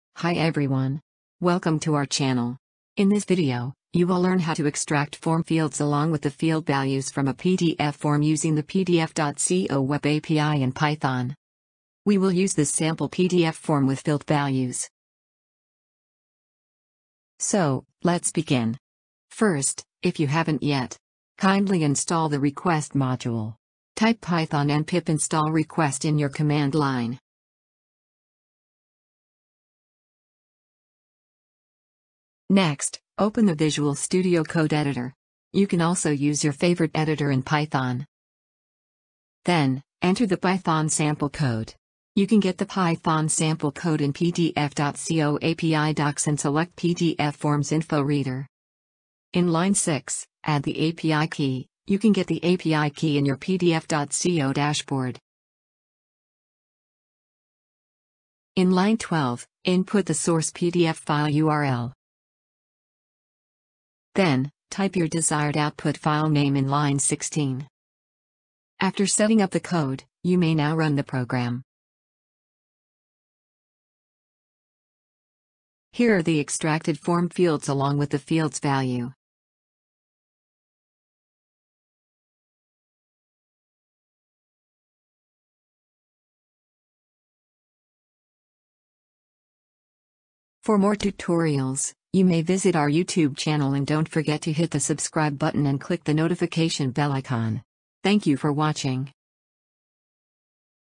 How to Extract PDF Form Fields in Python using PDF.co Web API
In this video, we'll demonstrate how to extract PDF form fields in Python using PDF.co Web API.